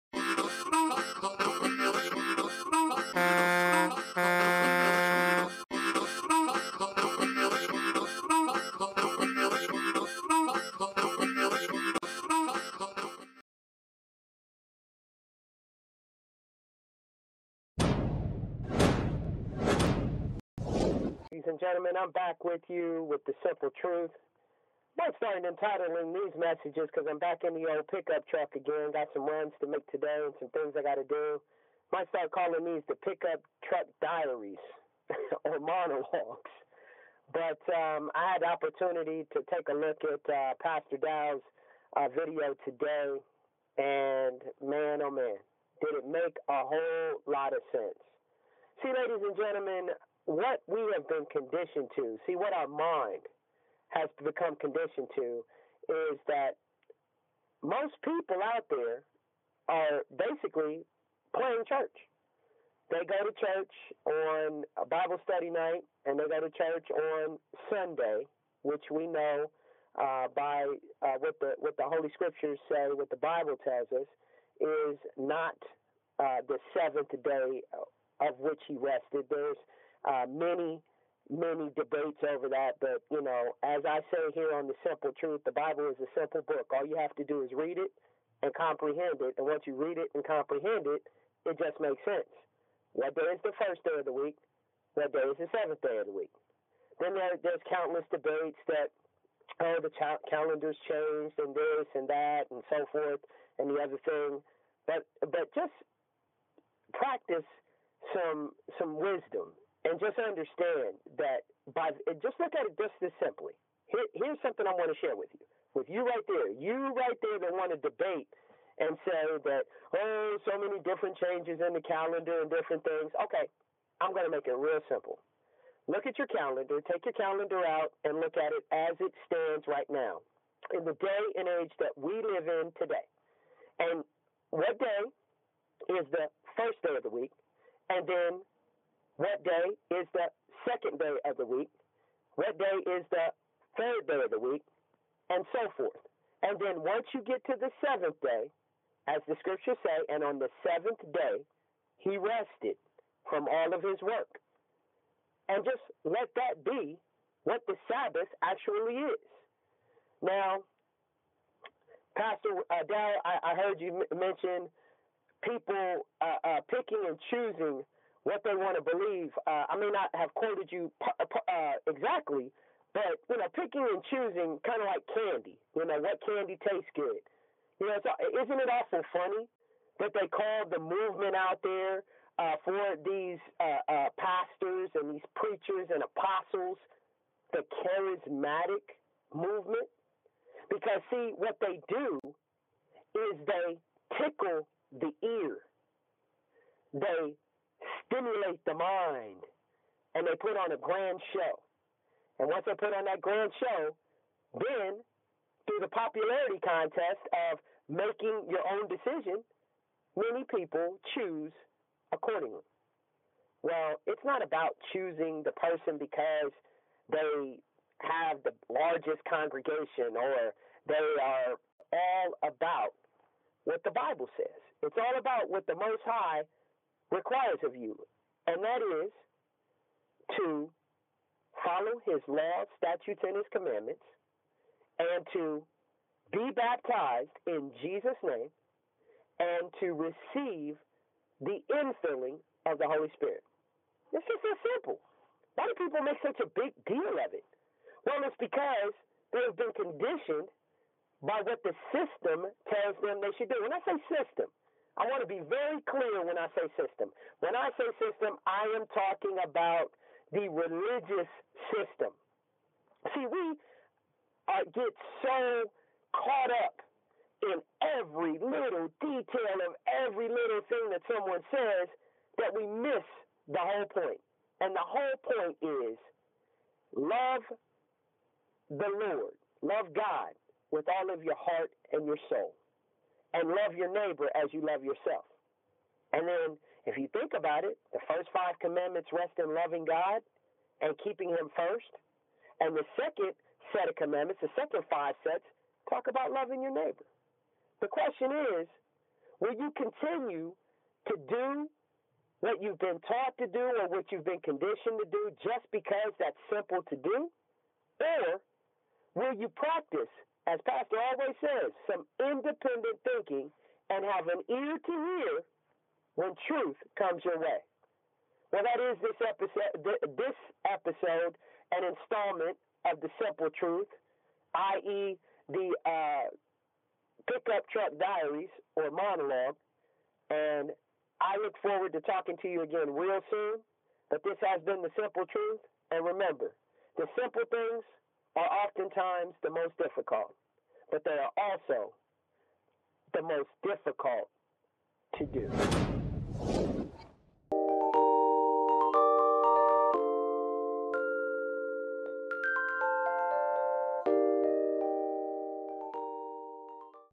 Pickup Truck Monologue #2: Response to Pastor Dowell's 'Religion'
This video 'PICKUP TRUCK MONOLOGUE' is in response to Pastor Dowell's video on 'Religion'. It is important heed the words and fruit of a TRUTHFUL pastor (Pastor Dowell) and NOT allow ourselves to be caught up in charisma, doctrine and who has the largest following.

The Simple Truth is...

-Follow God's LAWS, STATUTES and COMMANDMENTS
-Be baptised in the name of 'Jesus'
-Receive the Holy Spirit
-Heed the WORDS and FRUIT of a Jeremiah 3:15 pastor

Everything else will fall in to place!